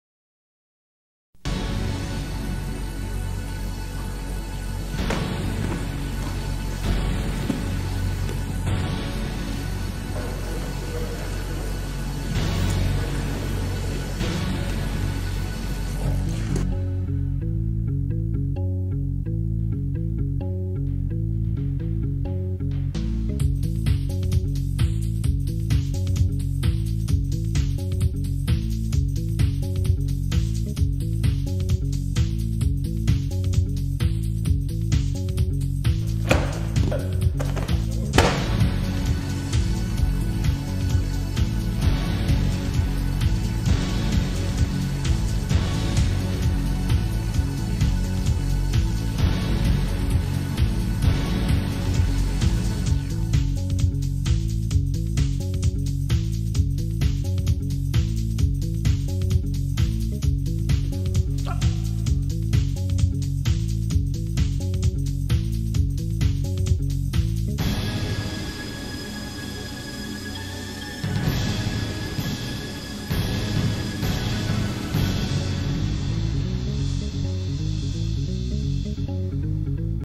Clothing as a Weapon - Official Trailer
Systema Russian Martial Art by Vladimir Vasiliev

Systema, a dynamic and practical fighting art of the special operatives, provides you with another brilliant weapon of self defense. It's the weapon for which you need no permits, no ammunition and no special training; you've been wearing it for years -- your clothing. Your best weapon is the one that's always close and convenient to apply, it seems harmless to the attackers, yet is unexpectedly powerful.

Learn instant takedowns, escapes, rolls and twists out of a jacket, ways to have clothing magnify the impact of pain compliance, strikes and chokes, conceal your punches, kicks and knife draws, implement your own and your attackers' clothing for smooth, calm and devastating control.

Whether you are a professional or civilian, expand your arsenal of options. Zip up your jacket and learn from the best.